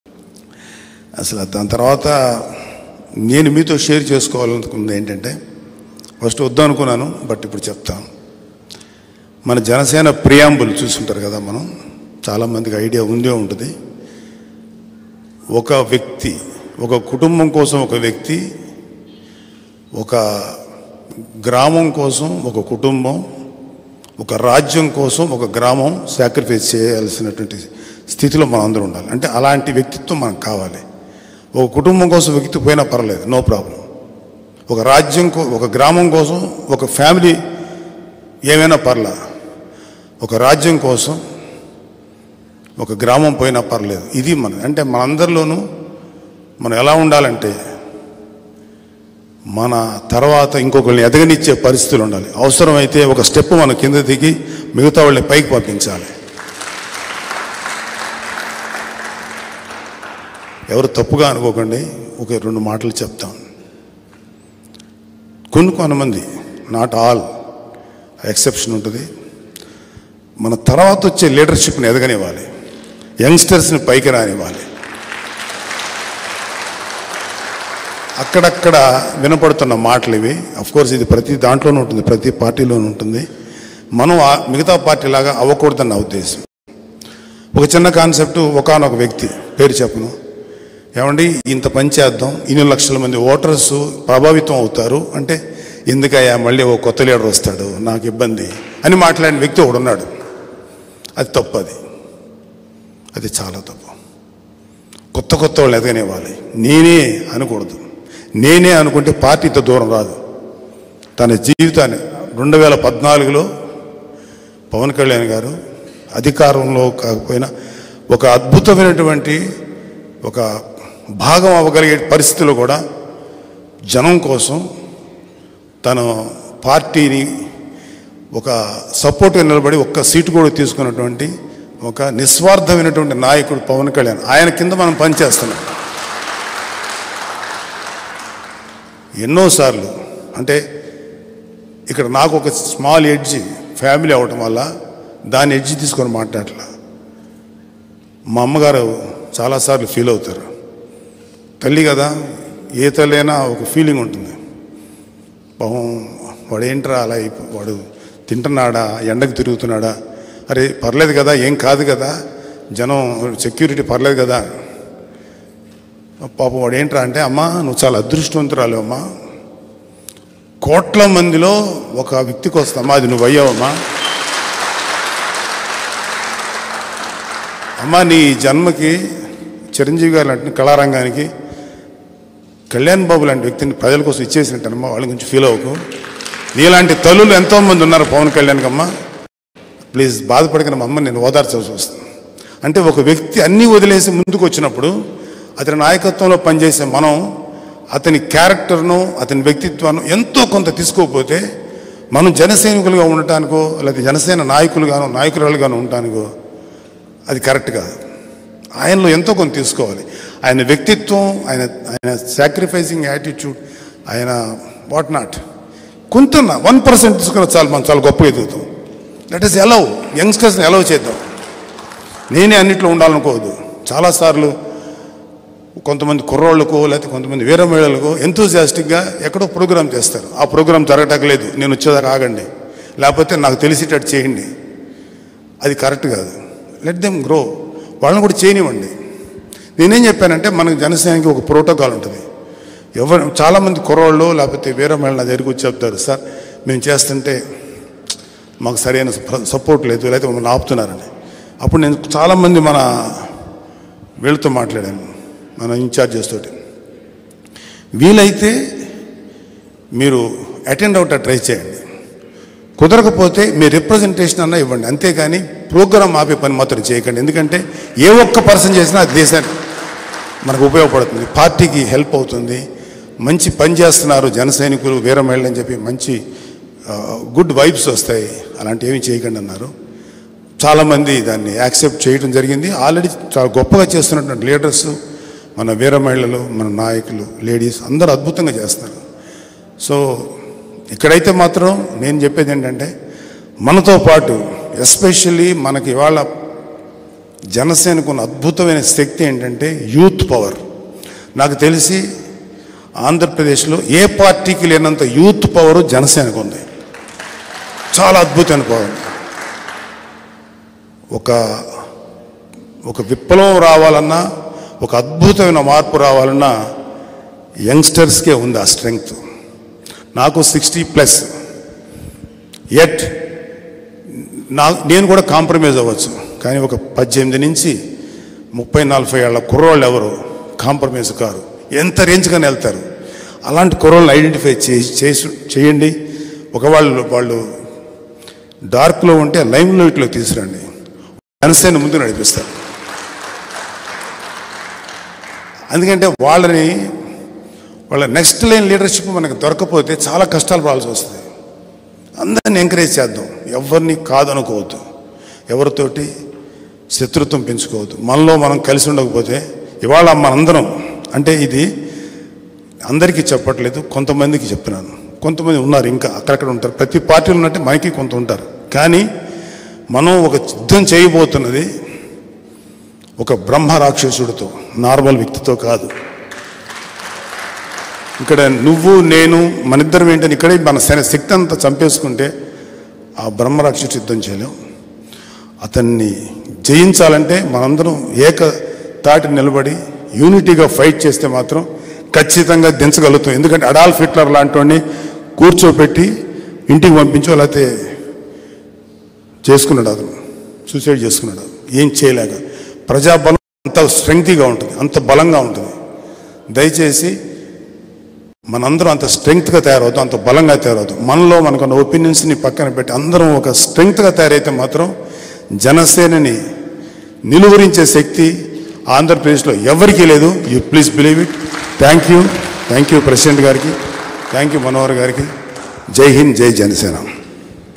మా అమ్మ అదృష్టవంతురాలు | Konidela Nagababu Emotional Speech About Pawan Kalyan & Chiranjeevi Garu
నువ్వు అదృష్టవంతురాలు అమ్మ అని చెప్తుంటా. Konidela Nagababu speech about Anjanadevi garu and Chiranjeevi Garu at Managalagiri party meeting.

Nagababu speech about welcoming new leaders and new generation leaders into party.

JanaSena party latest meeting updates and news.

JanaSena Varahi Yatra in Vizag starts from August 10th.

Varahi Yatra latest news and updates.